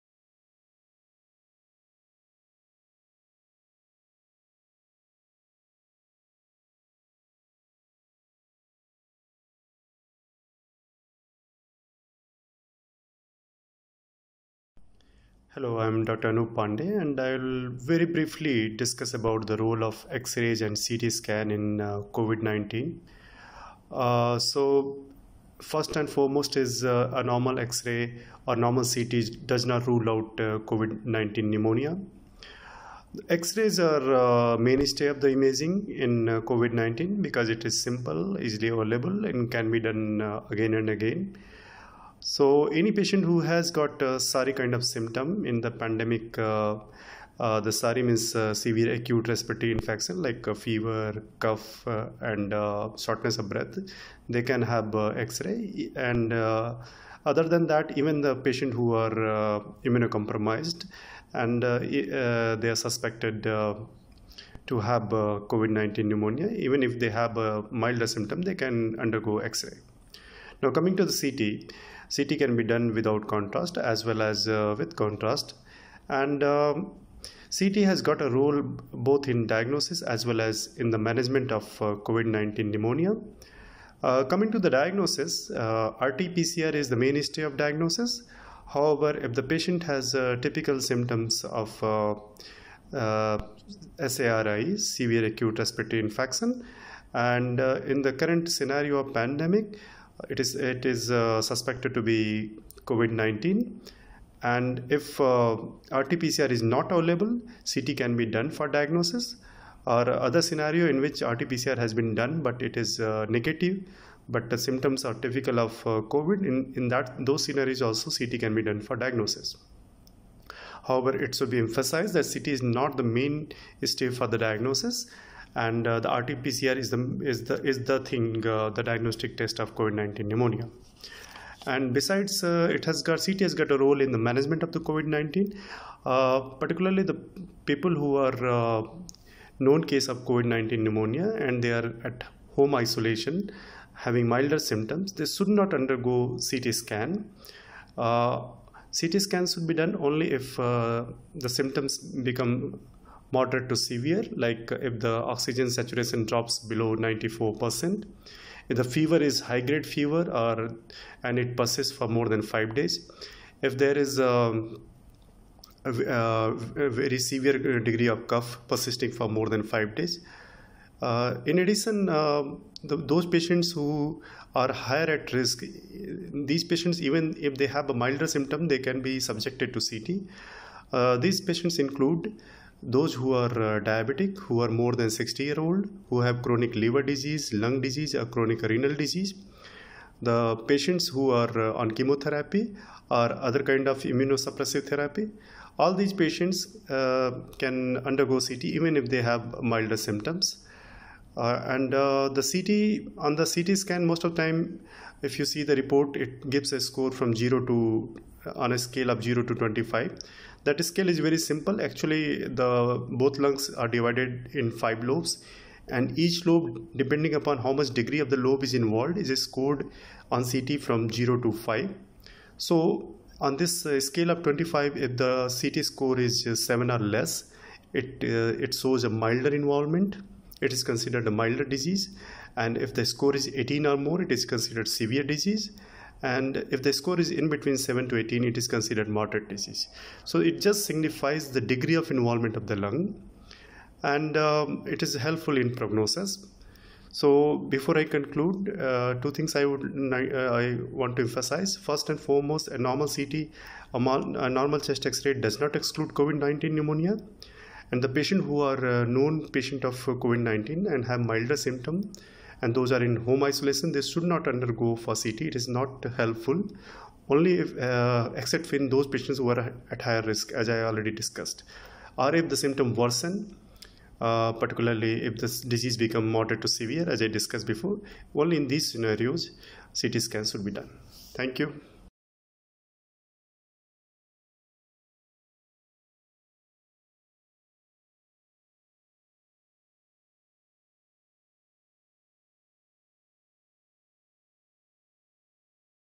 Role of CT Scanning in Coronavirus Diagnosis | CT Of Mild Covid Pneumonia By Dr. Anup Pandey
A brief description of the role of X-ray and CT scan in Covid-19(Coronavirus) by Dr. Anup Pandey (An invasive radiologist from Dubai). - Rgyan Wellness